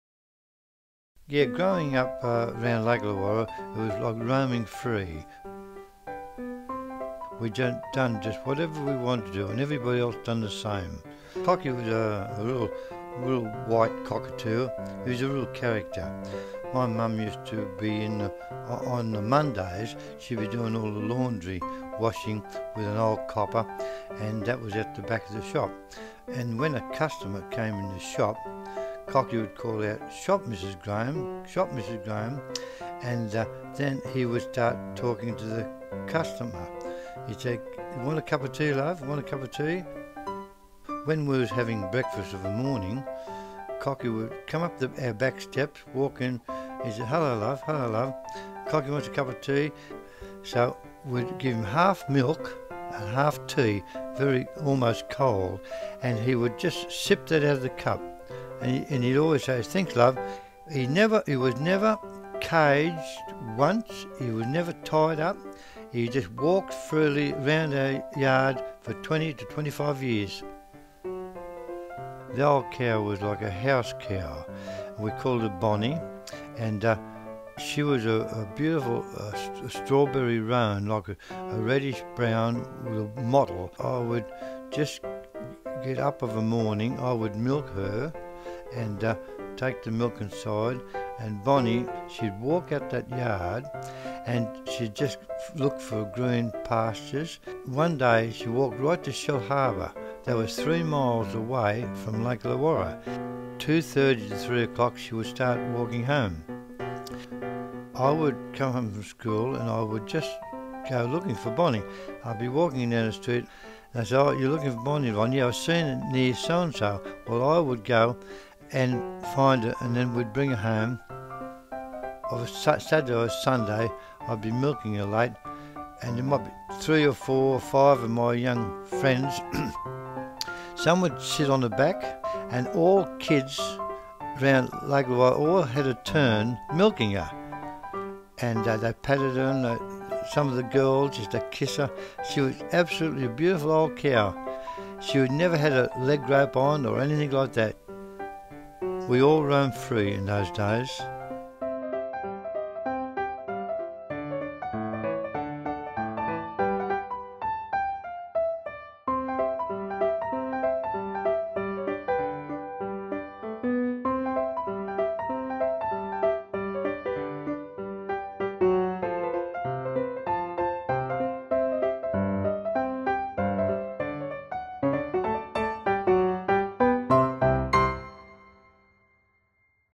Roaming Free - by Ron Graham (a digital story from the Lake Illawarra MAP Project)
MENTOR WRITER: Susan Kennedy
SYNOPSIS: Back in the day kids and animals just roamed free around Lake Illawarra.
SCRIPT: Growing up around Lake Illawarra was about roaming free.

We grew up with a cockatoo, Cocky, and an old cow, Bonny.  Cocky was a real character.  He used to keep shop for Mum when she was busy doing the laundry.  A customer would come by and hed call out, Shop, Mrs Graham!  And if she didnt come in fast enough hed be saying, Cup of tea, love?  One time he said it to the sanitary man who thought that it was mum offering him a cuppa.  And he said he wouldnt mind, thank you very much!

We never caged Cocky.  He had the run of the place and hed strut around the house and the yard chatting away in his beautiful voice.  He had a soft and lovely voice.  But he was a terrible show off.  Of course the kids all loved him.

The kids all loved Bonny too.  She was beautiful  - with big, brown eyes and a lovely colour.  All the neighbourhood kids used to come and have a go at milking her.  And shed let them sit on her back and go for rides.

The only problem was, she used to love to go exploring.  One minute shed be happily in the back yard eating the grass and then shed wander, wander, wander.  And every afternoon I had to go and find her.  Id be walking round the streets and everybodyd say, Are you looking for Bonny, Ron?  And Id say, Yeah.  And theyd say, We seen her up at so and so street.   And Id go up and find her.  One day I found her at Shellharbour.  And another day she was on Windang Island.  We all roamed free back in those days.